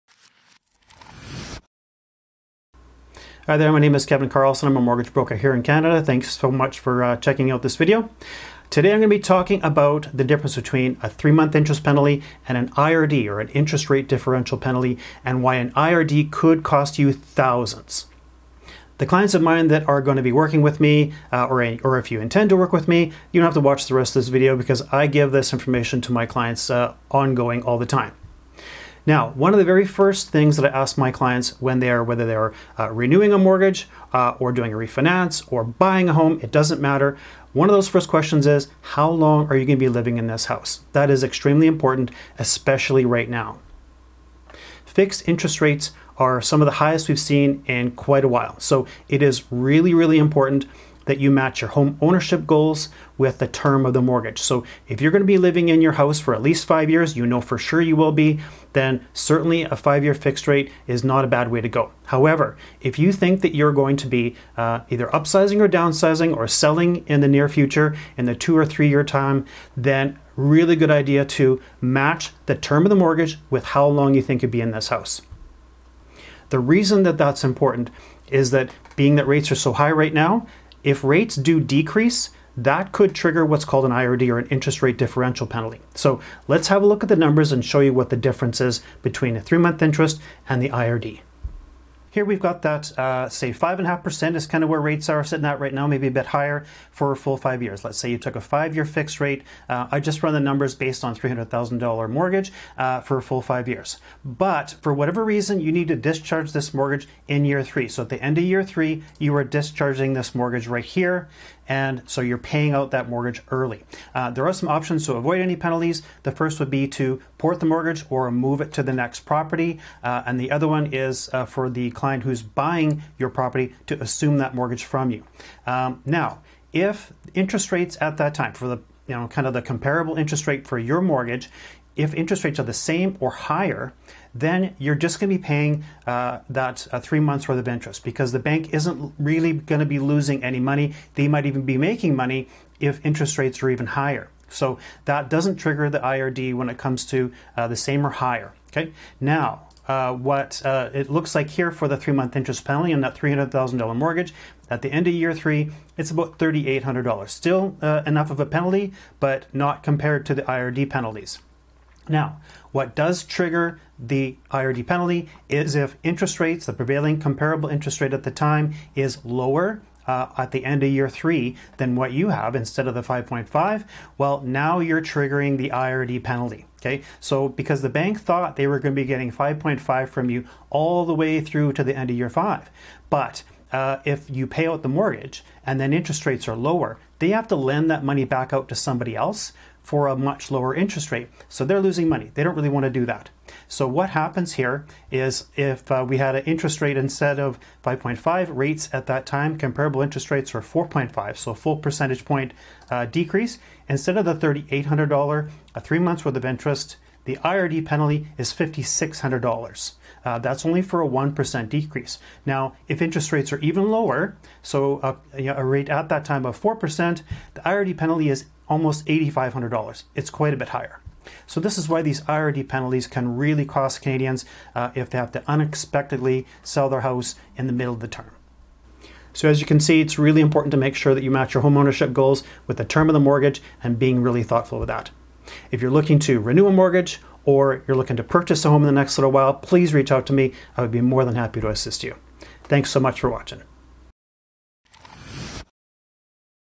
Mortgage Interest Rate Differential (IRD) Penalties Explained | Regina Mortgage Broker Kevin Carlson
Regina, Saskatchewan mortgage broker Kevin Carlson of Xeva Mortgage explains how early discharge penalties work with fixed rate mortgages in Canada.

Kevin Carlson - Mortgage Broker - FCAA Lic. 315902
Verico Xeva Mortgage - FCAA Lic. 509752